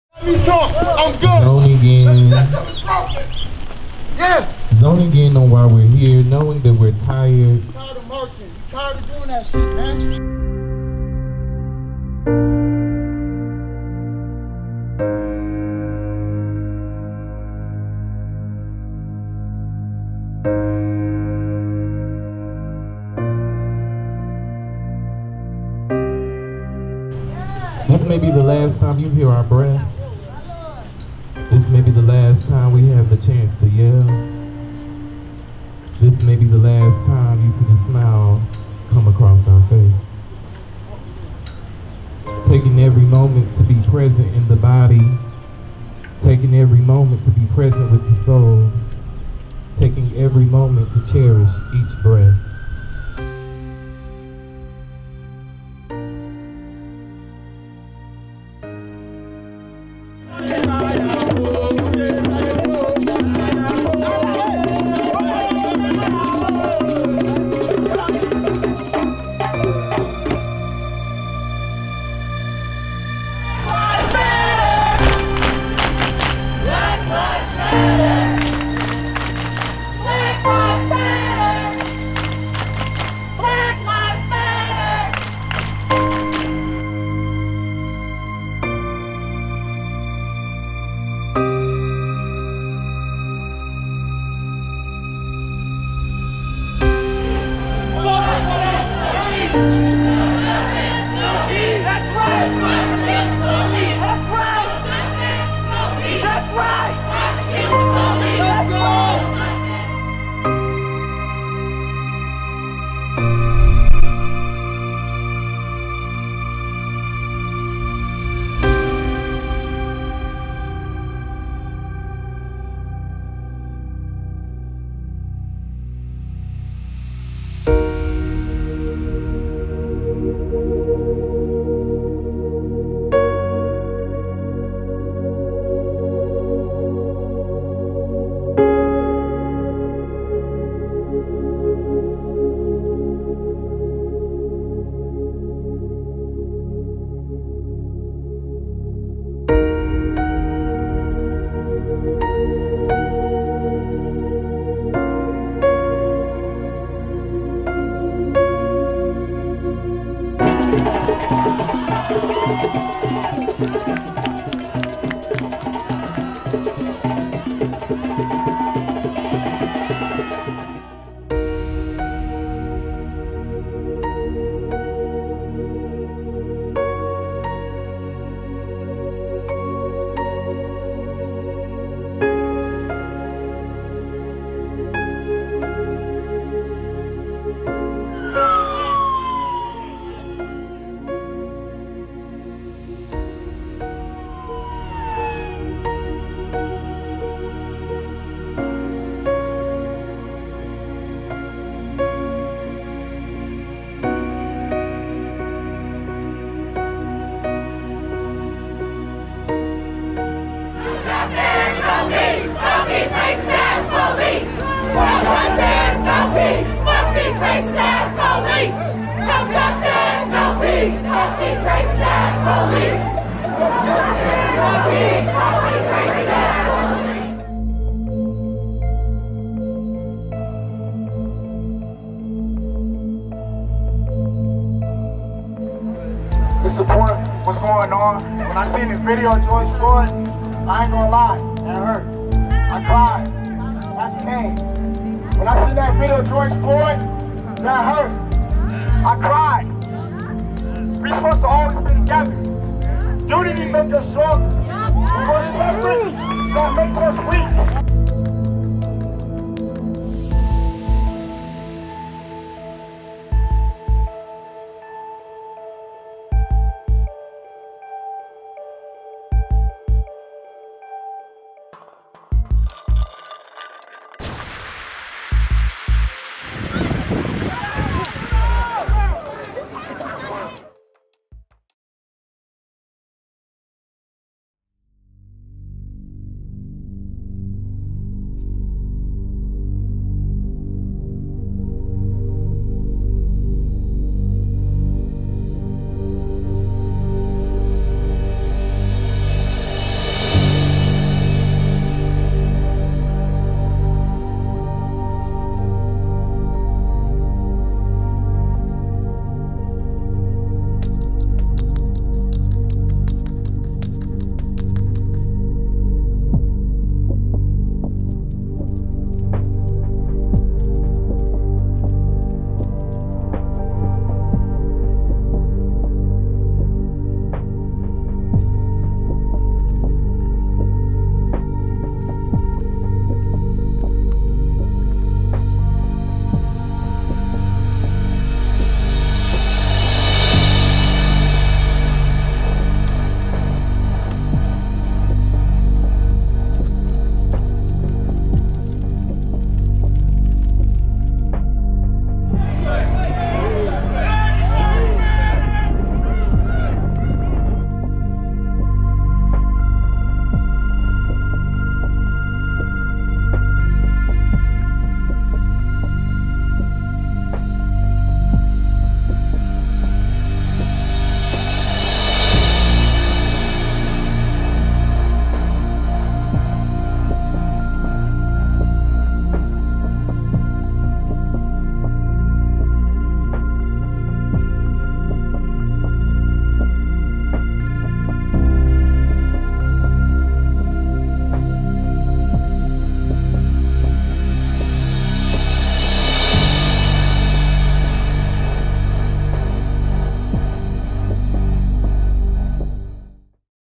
Black Lives Matter Rochester NY
The protest for George Floyd and Black Lives Matter started peacefully. And then later turned into a riot. There were two distinct events that happened on May 30th, 2020. I was there to support the movement and document. Towards the end, I had to stop filming to attempt to cover people from pepper bullets and my equipment.